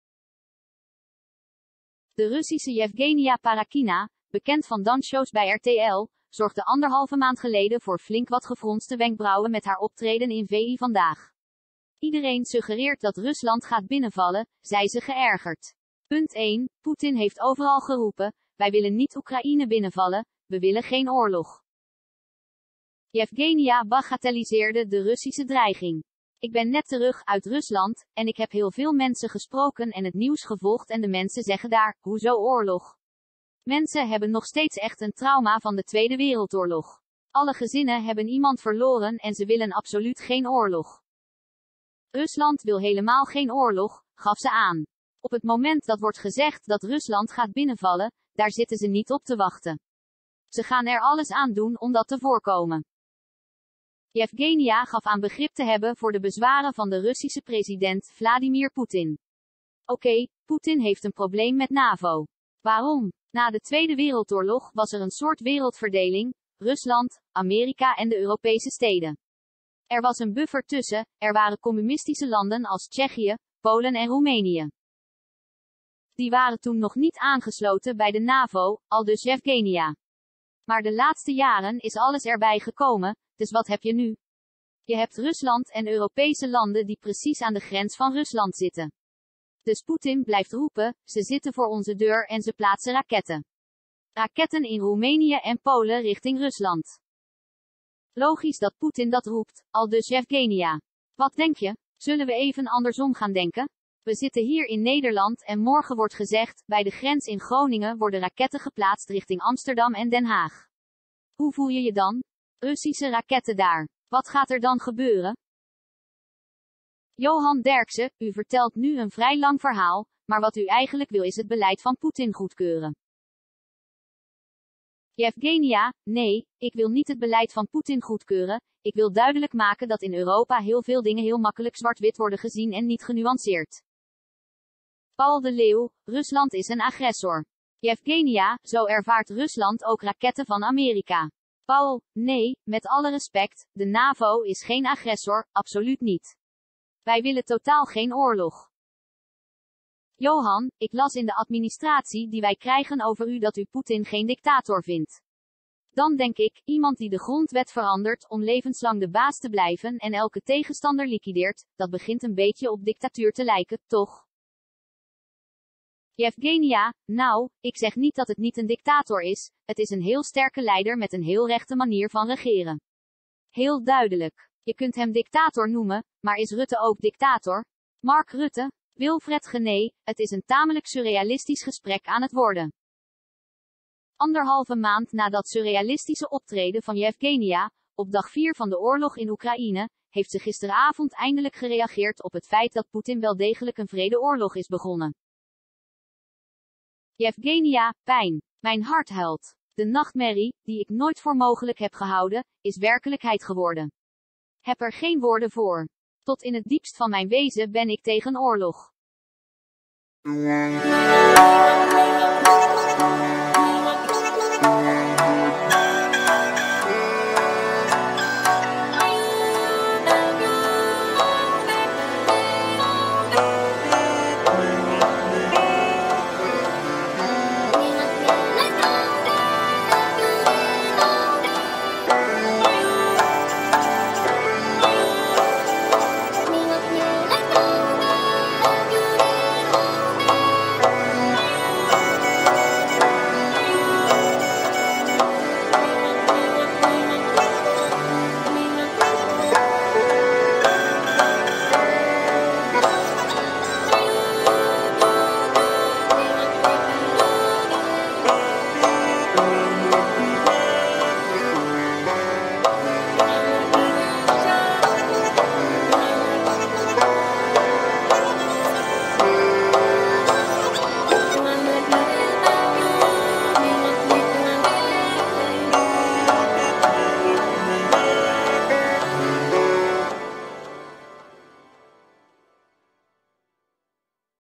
Euvgenia Parakhina na verdedigen Poetin bij VI: ‘Mijn hart huilt’
Euvgenia Parakhina heeft eindelijk gereageerd op de wrede oorlog in Oekraïne. Anderhalve maand na het verdedigen van Vladimir Poetin in de talkshow VI Vandaag, zegt ze nu dat haar hart huilt.